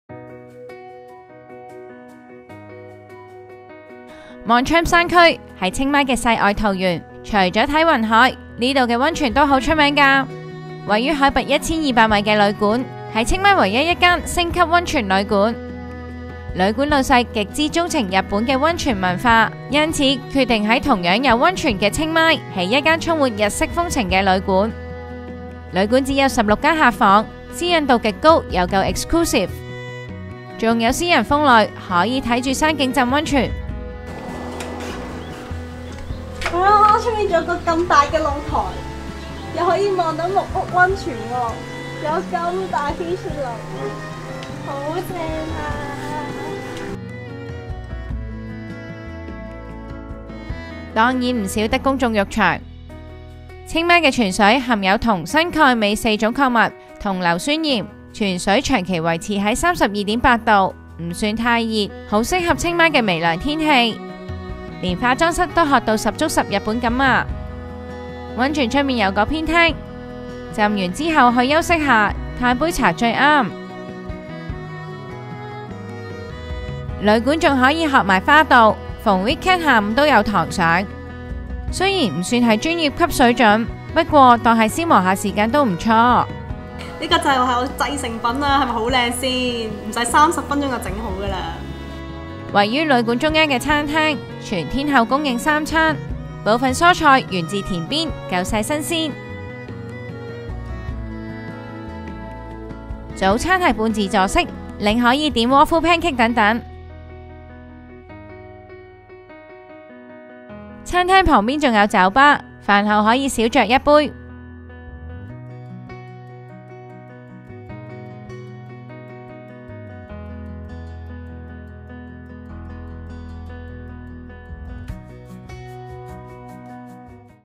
【清邁自由行】清邁唯一溫泉酒店 每晚HK$600起 夠貼地
Mon Cham 山區係清邁人的後花園。其中位於海拔 1,200 米的 Onsen @ Moncham，更是清邁唯一星級溫泉住宿。旅館只有 16 間客房，三面環山，房間面向山景，部分房設私人風呂。庭園、大堂、餐廳的裝修一樣充滿和風味。清邁的冬天最低約 15℃，微涼下浸溫泉也不錯。

【info】
Onsen @ Moncham
地址：293 Moo 2 Tambon Pong Yaeng, Amphoe Mae Rim, Chiang Mai, 50180
交通：市中心乘車約 1.5 小時

全方位緊貼《U Magazine》社交平台